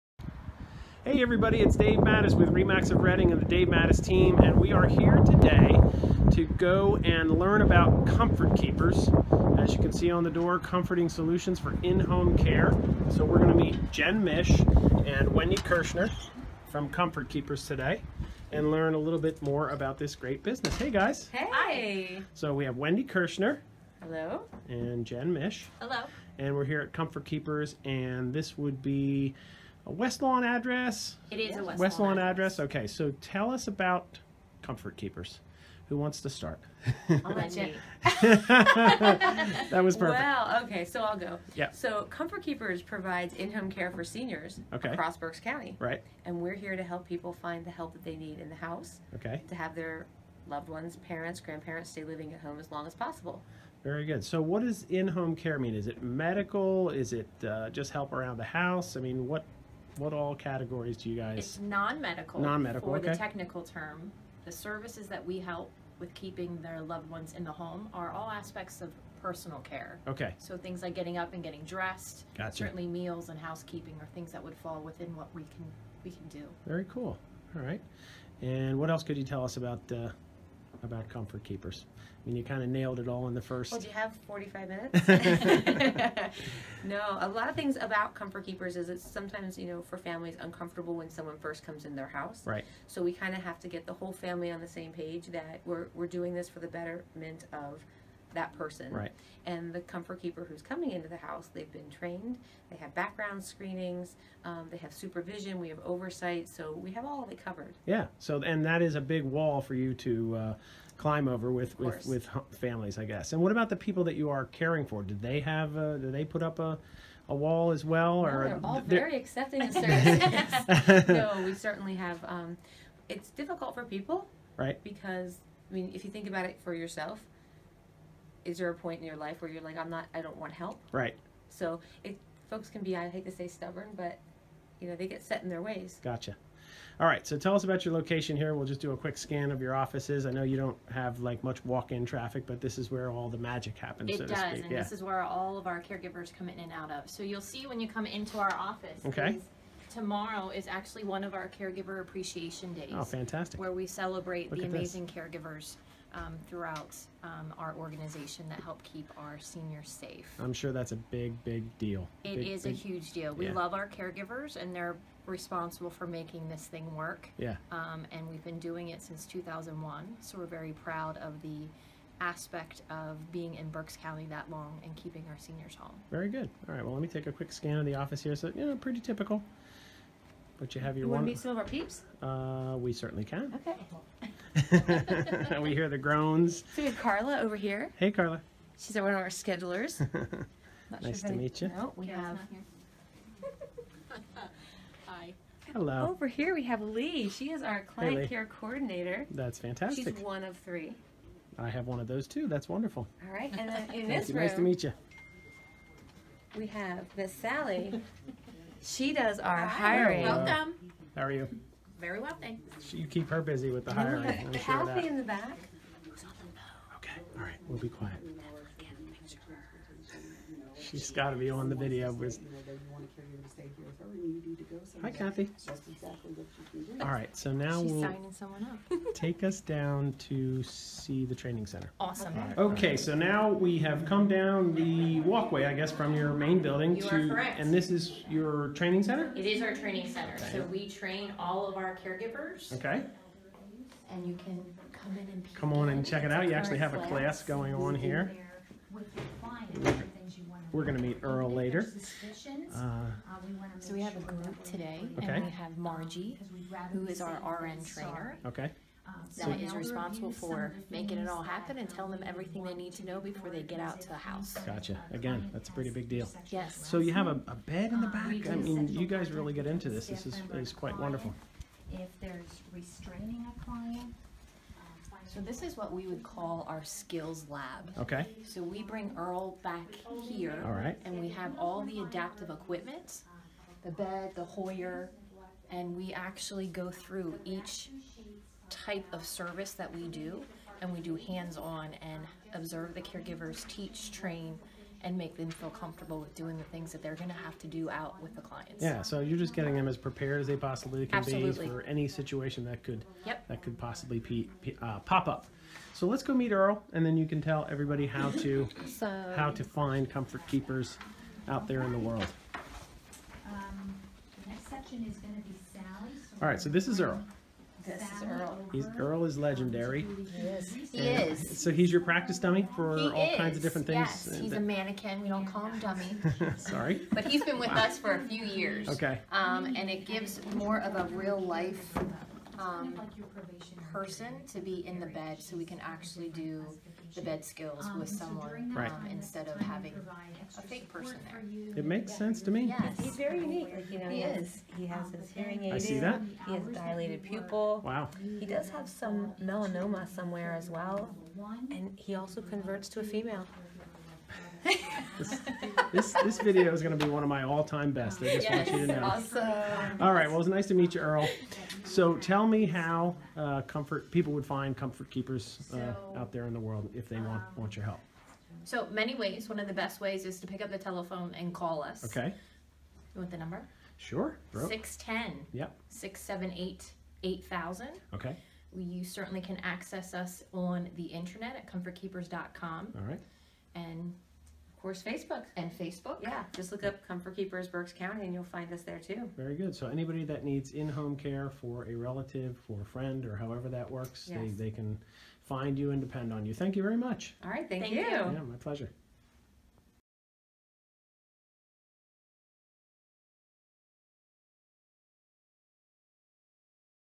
Local Business Spotlight - Comfort Keepers
In my latest local business spotlight, I went to Comfort Keepers to learn about everything they do for seniors in the Berks County area.